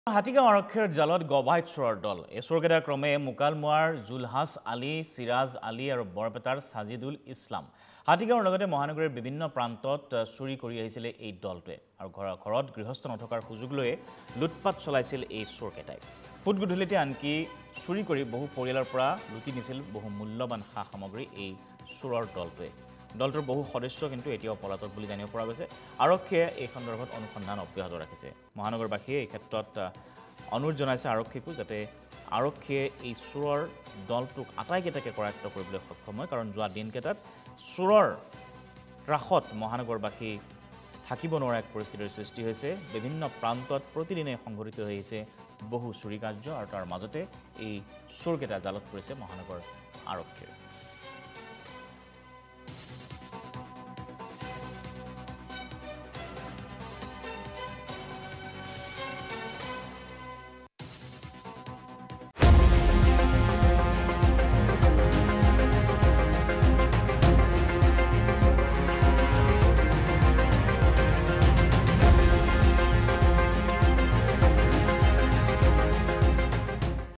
হাতীগাঁও আৰক্ষীৰ জালত গভাইত চোৰৰ দল
▶️ মহানগৰীৰ ভিন্ন প্ৰান্তত চুৰি কৰিছিল দলটোৱে
▶️ ঘৰত কোনো নথকাৰ সুযোগ লৈ চলাইছিল লুটপাত